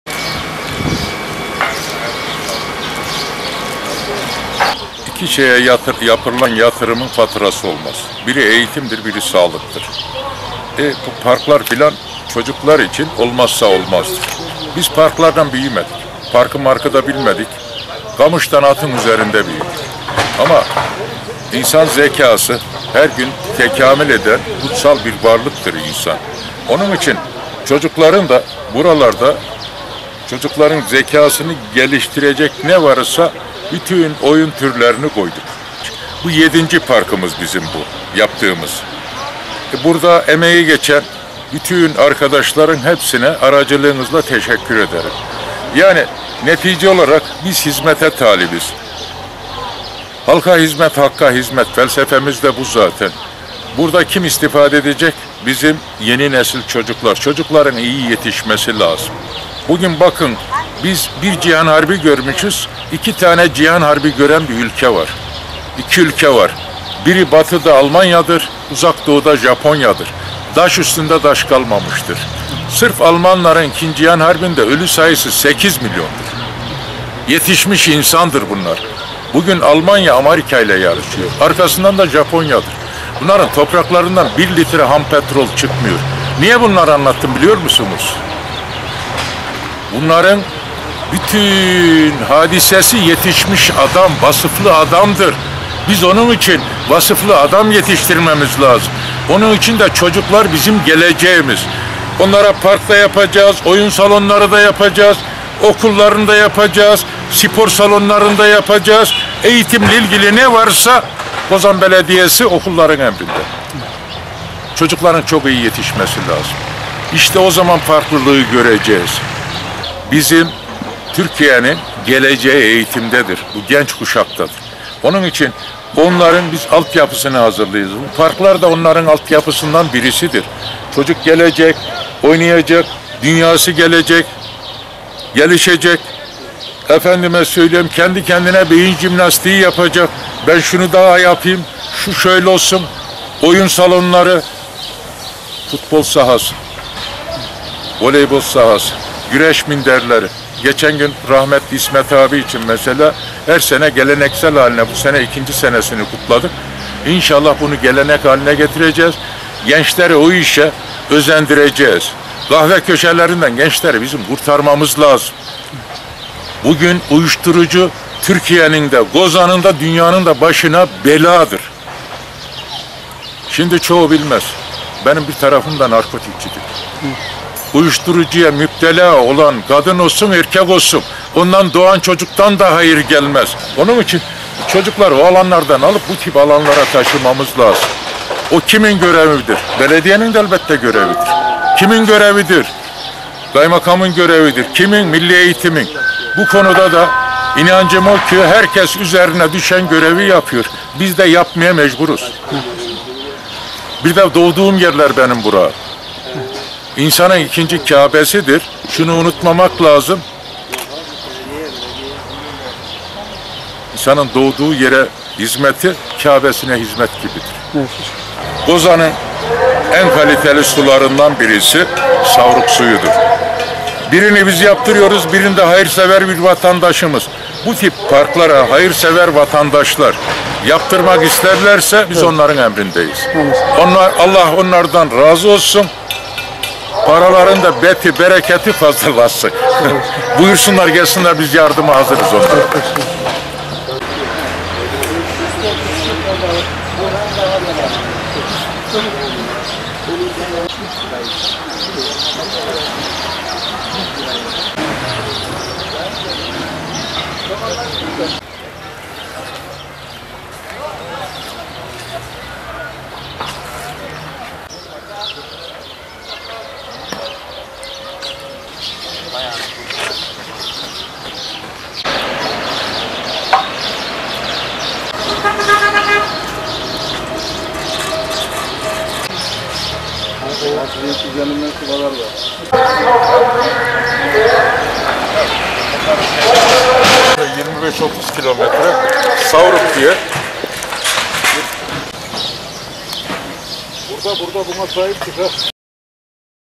Cumhuriyet Parkı Çalışmalarımızda sona gelindi
Belediyemizin 'Parklar yeniden hayat buluyor' projesi kapsamında, yaklaşık 25 yıl önce yapılan Cumhuriyet Parkında başlattığı yenileme ve düzenleme çalışmasında sona yaklaşıldı.

Uzun yıllar bakımsızlık nedeni ile vatandaşların ilgi göstermediği Cumhuriyet Parkında başlatılan yenileme çalışmalarını inceleyen Belediye Başkanımız Musa Öztürk, park içerisinde çocuk oyun gruplarının yanı sıra yetişkinler için mescit, amfi tiyatro, anneler için emzirme odası, kafeterya, fitness alanları, yürüyüş parkurları, süs havuzu gibi birçok sosyal alanın olacağını söyledi.

Güçlü bir Türkiye için iyi eğitim almış ve çocukluğunu iyi yaşamış vasıflı insanlara ihtiyaç olduğuna dikkat çeken Başkanımız yaptığı açıklamada şu ifadelere yer verdi; “Baştan sona yenileyerek modern bir hale getirdiğimiz Cumhuriyet Parkımıza yavrularımızın ihtiyacı olan ve zekâlarını geliştirecek birçok oyun grubu yerleştiriyoruz. Bu park göreve geldiğimiz günden bu yana vatandaşlarımızın istifadesine sunduğumuz yedinci parkımız olacak. Güçlü bir Türkiye için bizimde iyi eğitim almış ve çocukluğunu iyi yaşamış vasıflı insanlara ihtiyacımız var. Çocuklar bizim geleceğimiz onların iyi yetişmesi için parklar, okullar, oyun alanları, spor sahaları kısaca eğitimle ilgili ne varsa yapacağız. Gençlerimizi kahve köşelerinden ve uyuşturucu belasından kurtararak, böyle güzel yapılmış parklara ve sosyal aktivite yapabilecekleri farklı alanlara kaydırmamız gerekiyor. Bunda diğer kurumlara olduğu kadar belediyelere de görev düşüyor. Yaptığımız bu parklar güçlü bir neslin yetişmesi ve gençlerin kötü alışkanlıklara bulaşmasını engelleyecek altyapılardan sadece birisi. İnanıyorumki diğer kurumlarda üzerine düşenleri yapıyordur.”

Yenilenen parkta hayırsever bir vatandaşın hayrat çeşmesi yaptırdığını ifade eden Başkanımız, diğer parklarda çeşme yaptırmak isteyen hayırseverlere da imkân vereceklerini sözlerine ekledi.